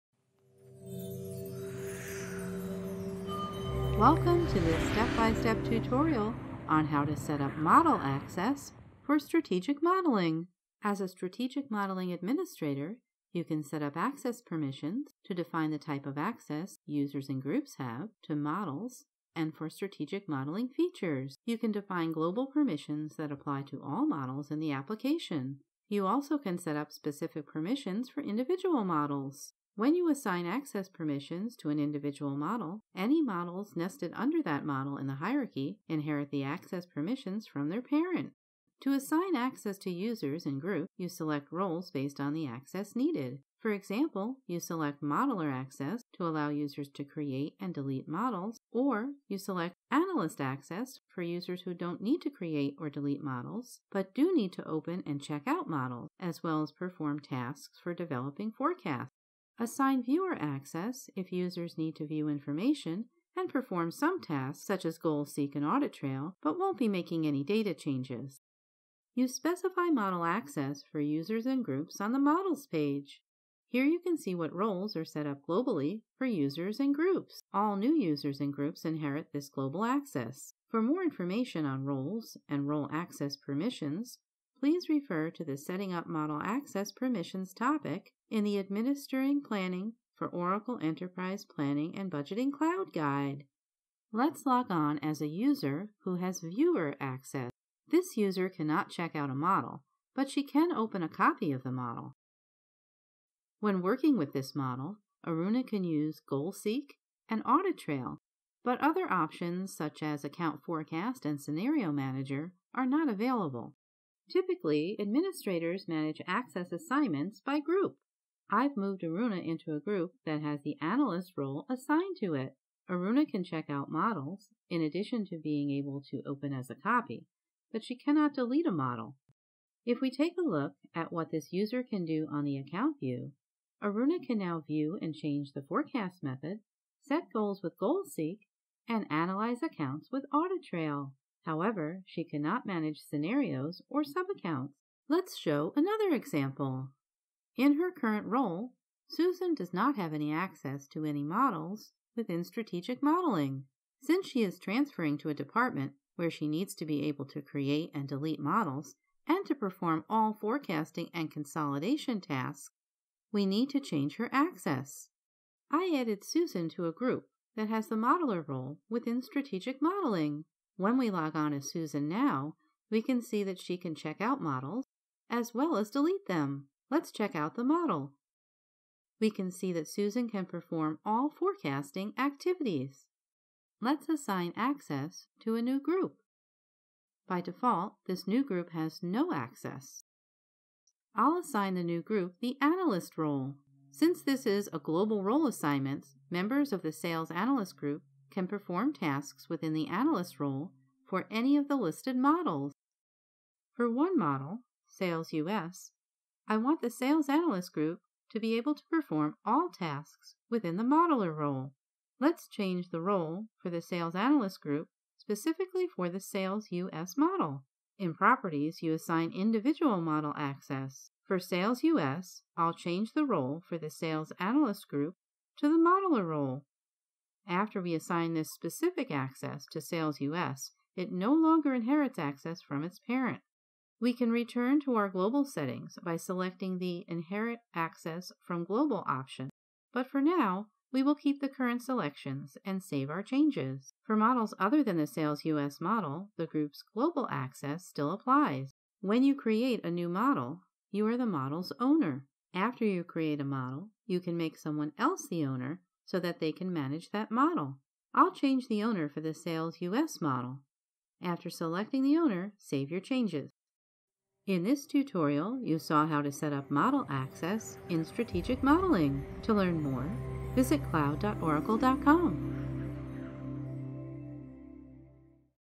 Setting Up Model Access for Strategic Modeling in Oracle Enterprise Planning Cloud
This tutorial demonstrates how to set up model access for Strategic Modeling. You start by reviewing Global access which is the access that all new users and groups inherit. You review the access defined for creating and checking out models, and for performing tasks within models for each of the Viewer, Analyst, and Modeler roles. You change the default access for a new group, and you also assign model-specific access for a group.

Enterprise Planning and Budgeting Cloud is now Planning, a business process in Oracle EPM Cloud